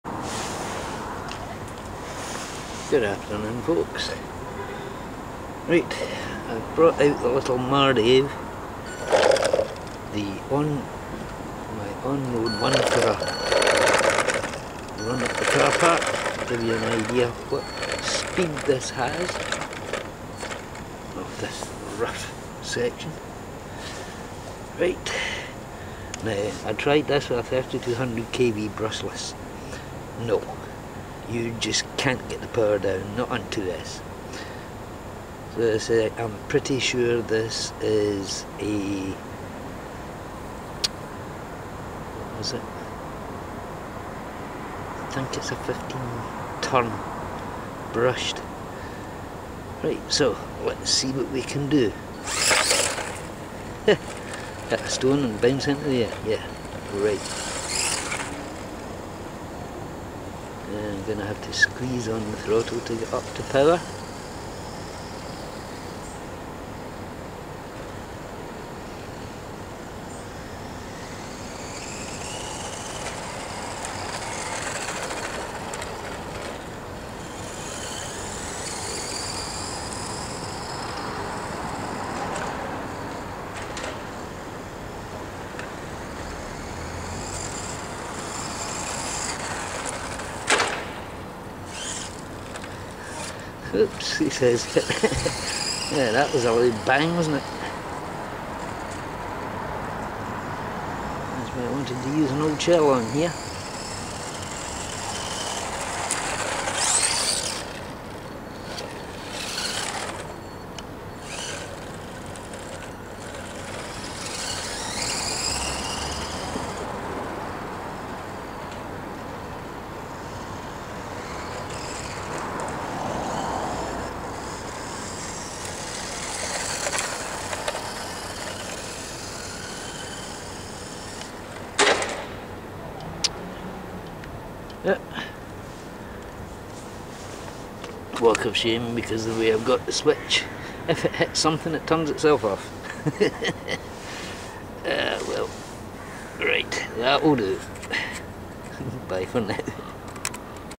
Mardave V12 running vid.
Just a short outdoor test. Needless to say,things didn't quite go as planned. The second crash dislodged everything off the chassis and even pulled the motor wires apart.